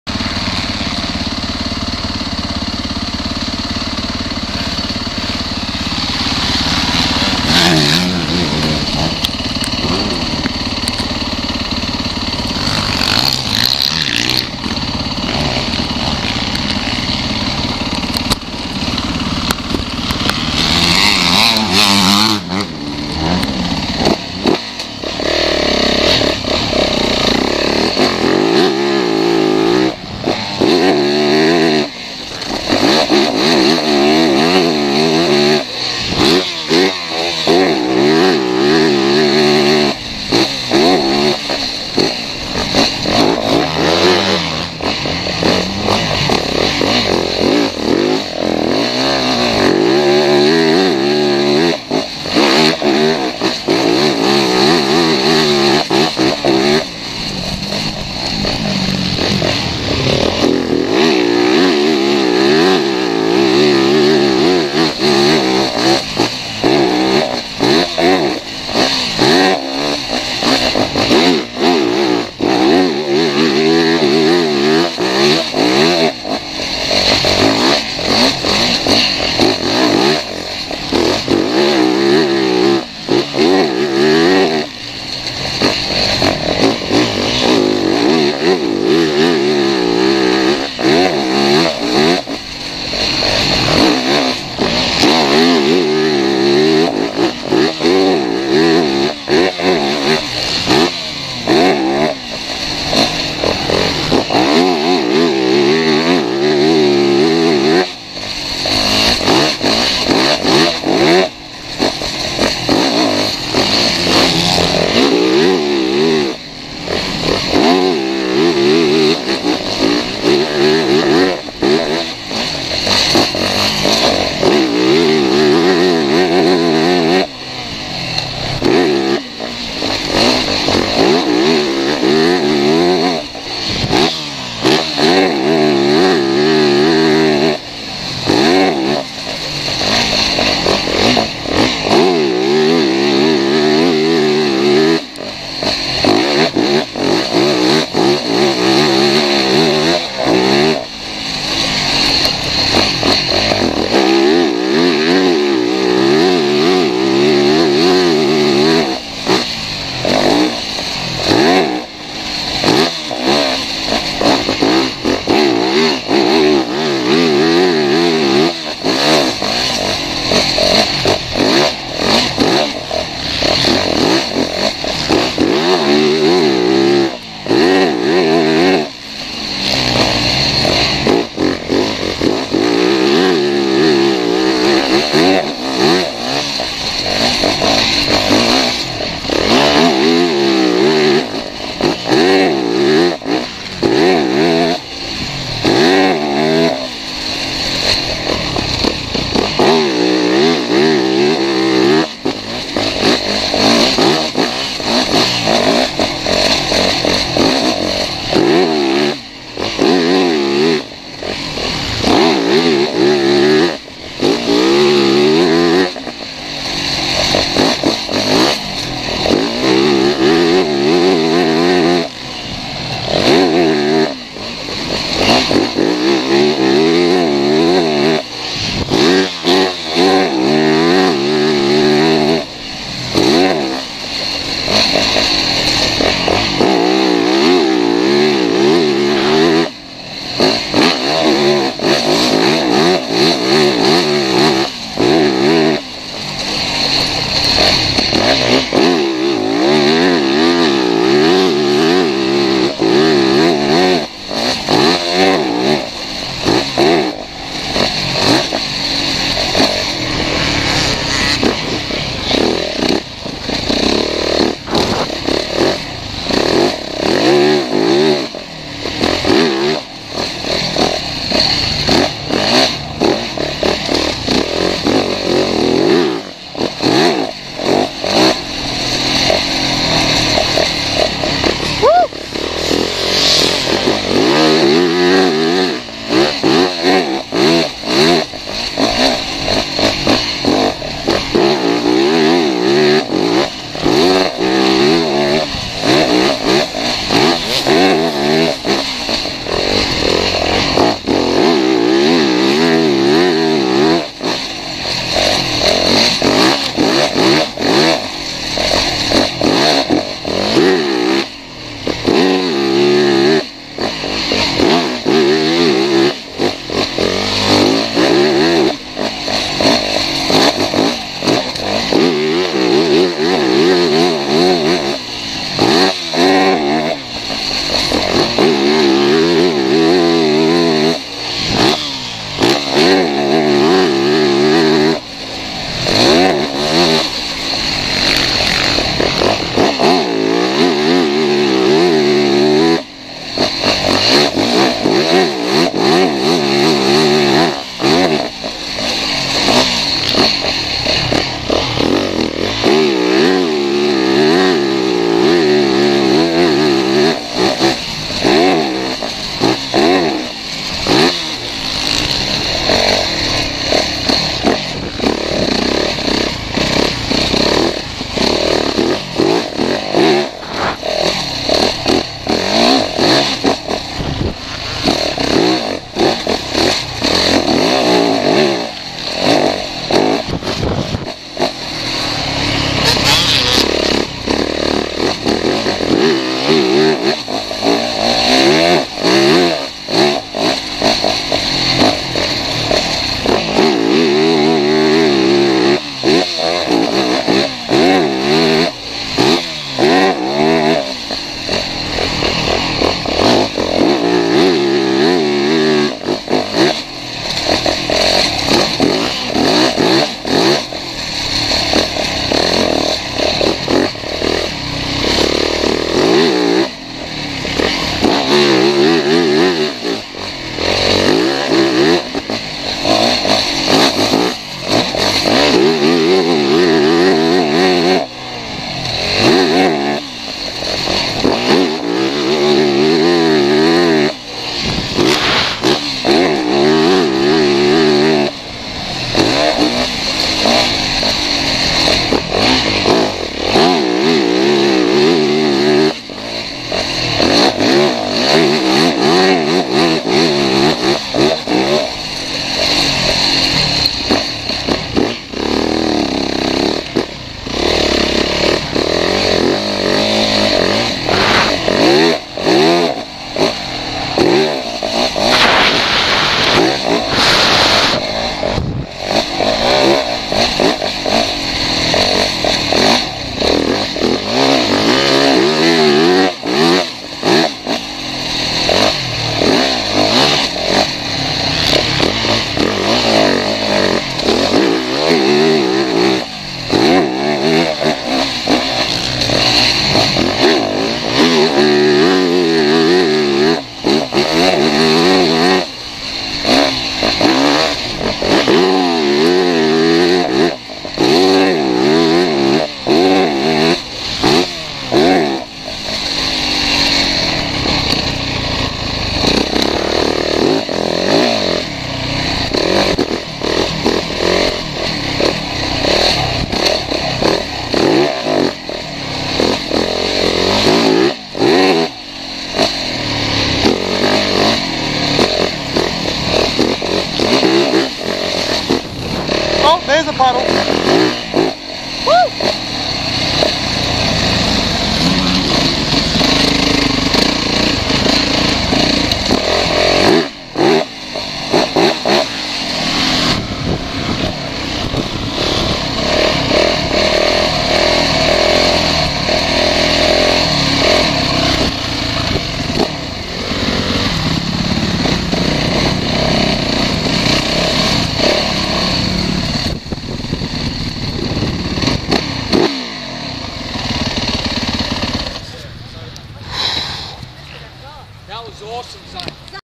Impact Series HD Video Goggle - Off Road Track
Liquid Image Impact Series HD - off-road/mx Video Goggle model 365. HD 720P video recorded in Australia.